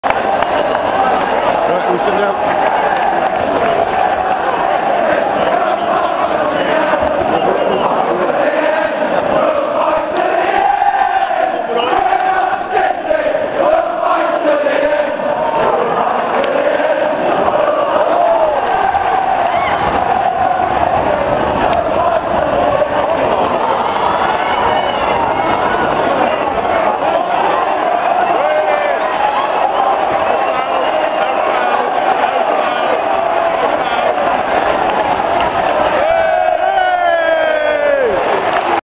Fulham again
City fans the best in the land and all the world. At Fulham, 29 Sept 2012